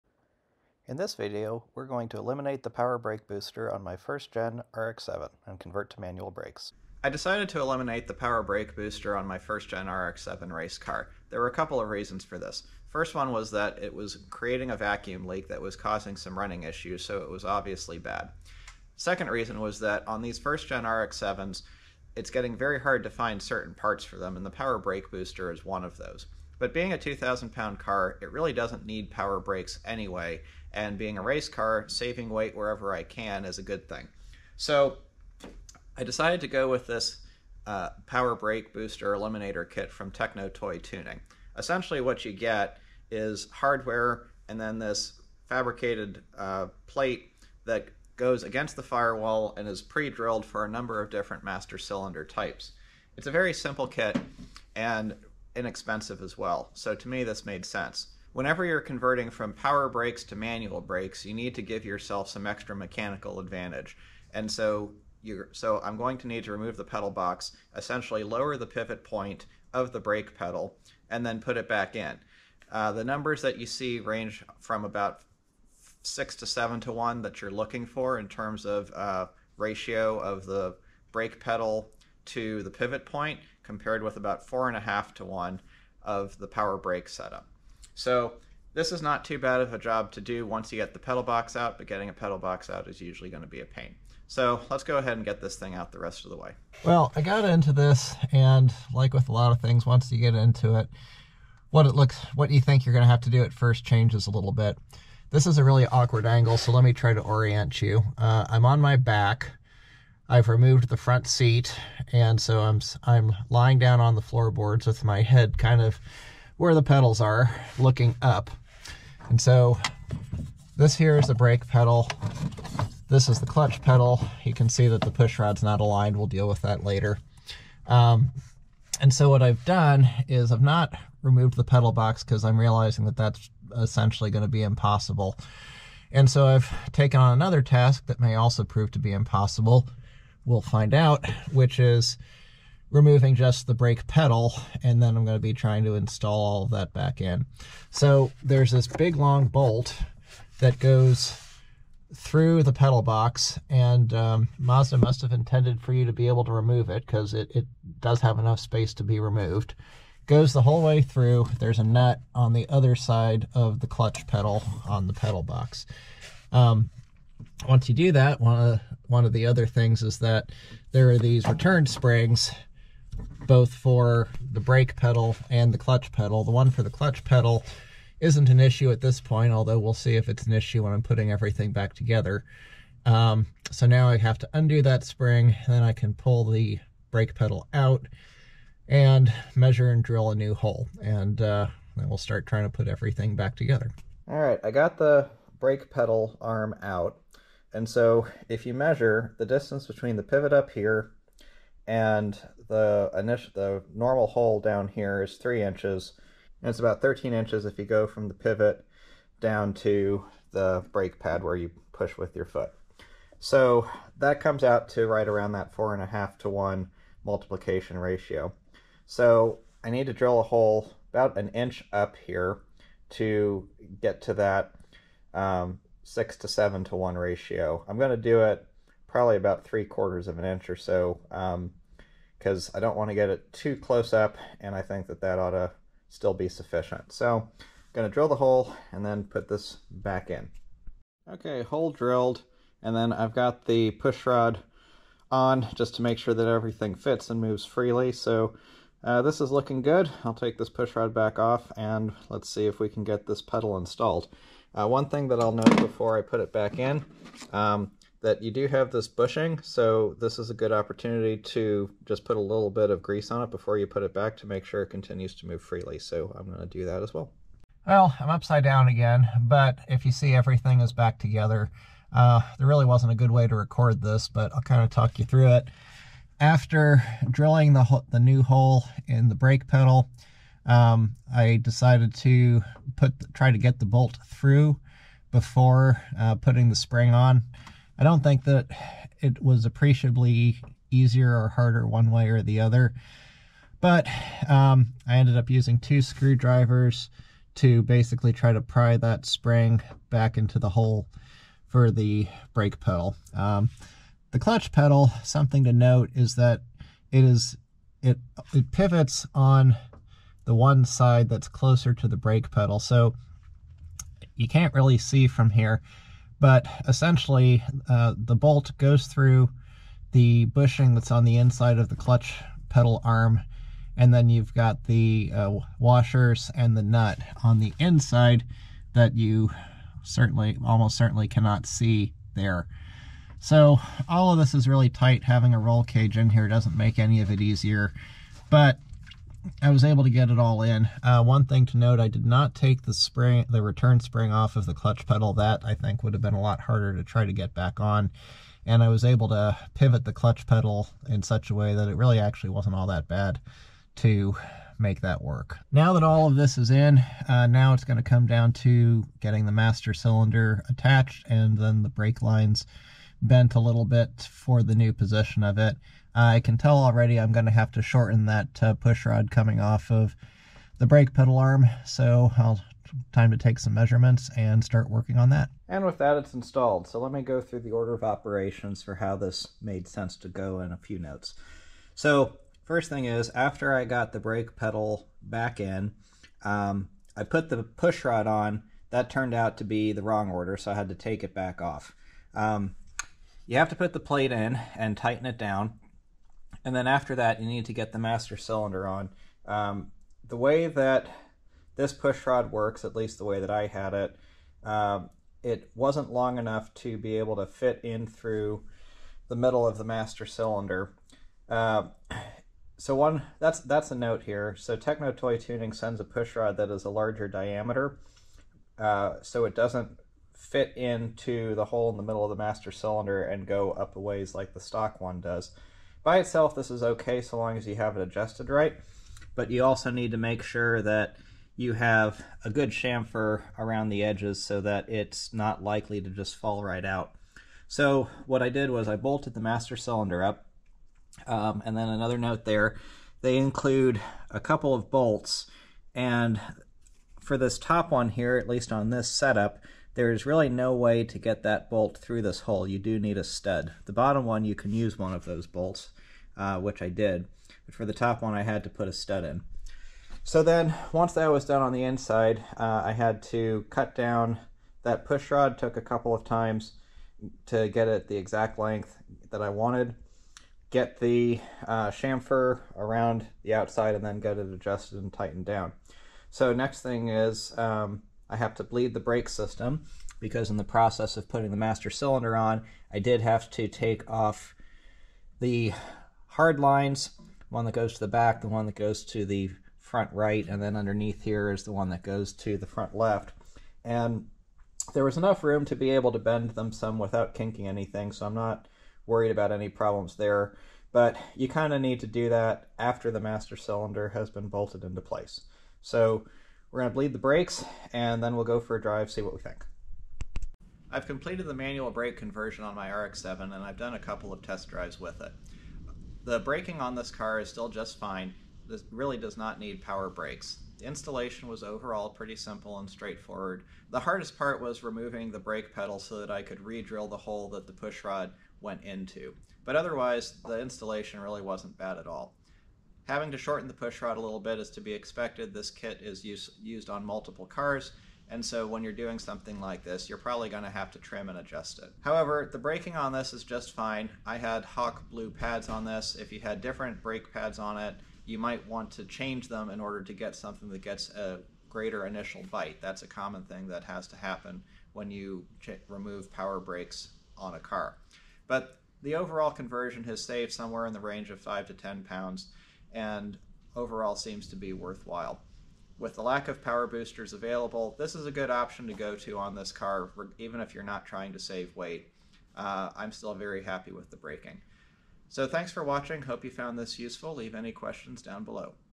First Gen Mazda RX-7 (FB) Manual Brake Conversion
Links to the Hawk Brake pads I use on my RX-7 with the manual brakes:

This Video Shot with a GoPro Hero 11

The power brake booster had failed on my 1st gen RX-7. I couldn't find a replacement, and a 2,000 lb car doesn't need power brakes anyway. So, I decided to convert to manual brakes using a kit available from Techno Toy Tuning.